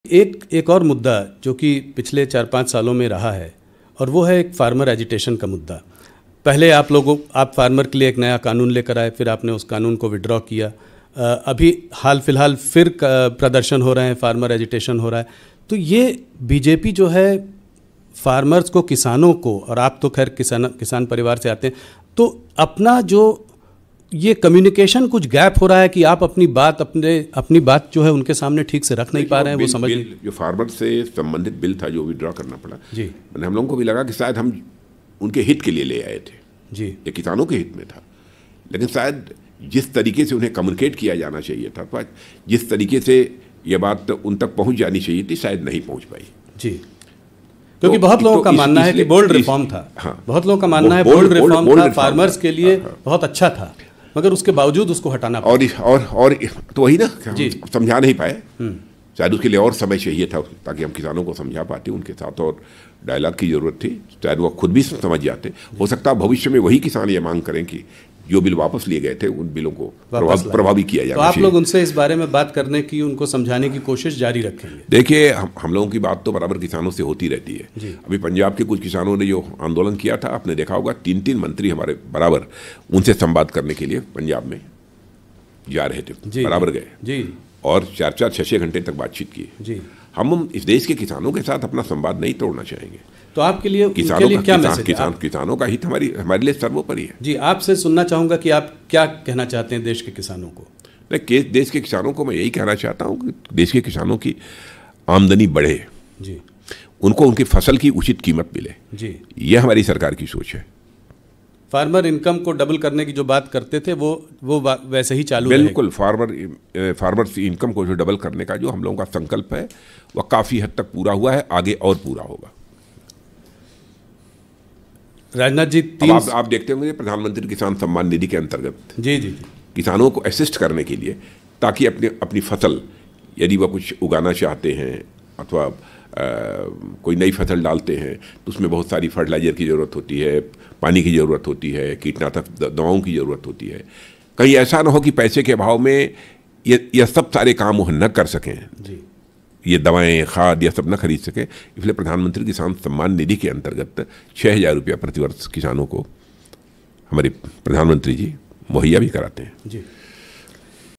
Rajnath Singh Exclusive Interview: किसानों के आंदोलन पर क्या बोले राजनाथ सिहं?।Rajneeti। News 18 UP
लोकसभा चुनाव 2024 से पहले NEWS18 के एडिटर इन चीफ़ राहुल जोशी के साथ रक्षा मंत्री राजनाथ सिंह की बेबाक बातचीत. देखिए पूरी खबर...